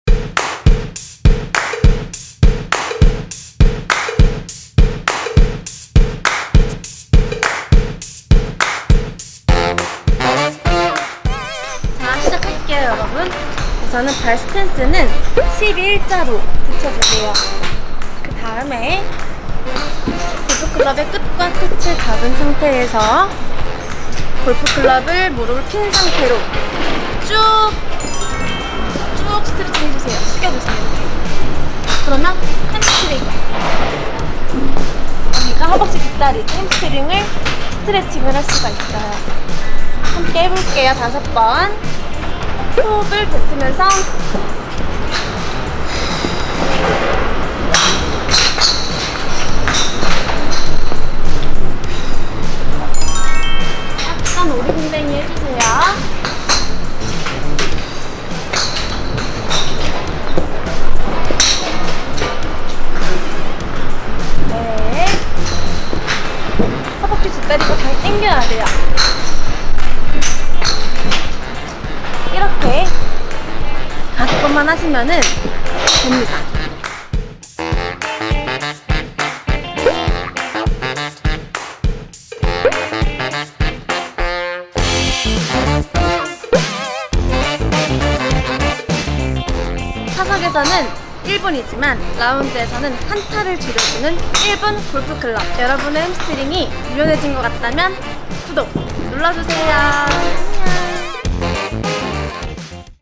[1분골프클럽] 골프연습장 에서 진짜하는 골프스트레칭 햄스트링 스트레칭편 l
[1분골프클럽]

2020 XGOLF 홍보모델 최설화의 1분 골프클럽 😘⛳️
골프스트레칭 햄스트링 스트레칭편

 👥 👥 여러분 👥 👥
아직도 그냥 스트레칭하고 연습하세요?
아니면,
스트레칭도 안하고 연습하시는건가요??? ⛳️

세상에 🤦🏻‍♀️🌿

연습전에 골프클럽을 활용한 스트레칭으로
우리 함께 1타를 줄여보자구요 🏌🏻‍♀️